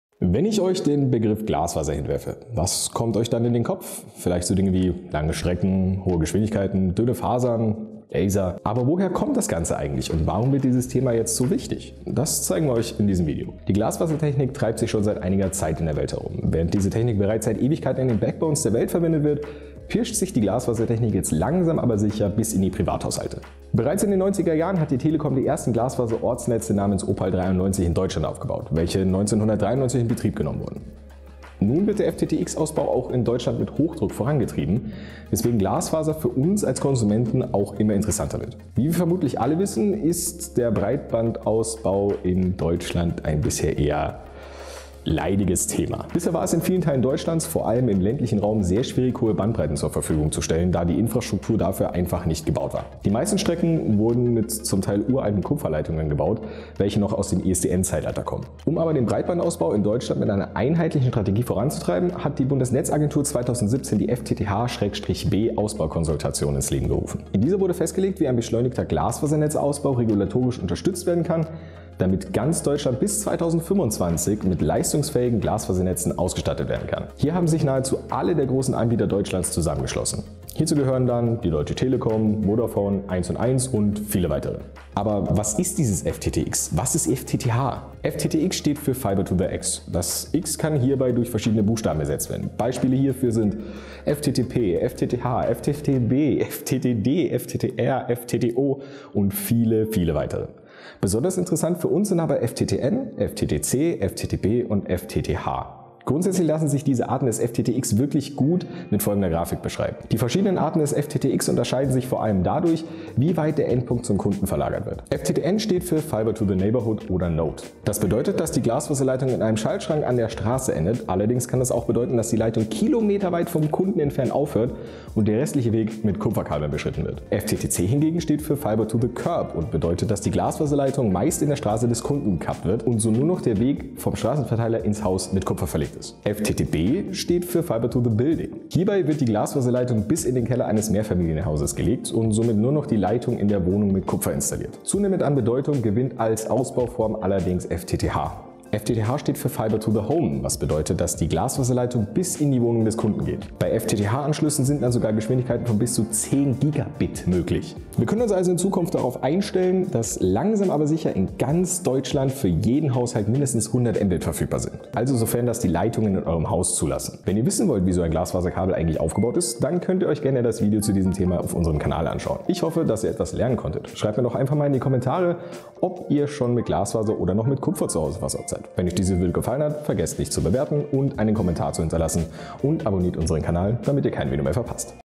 Was ist FTTx? | Glasfasereinstieg mit KWS
Was ist dieses FTTx? Das zeigen wir dir in diesem Video!

• KWS Electronic Test Equipment GmbH: Entwicklung, Herstellung, Vertrieb und Service: Alles aus einer Hand
• Von SAT bis OTDR, wir helfen Ihnen gerne weiter.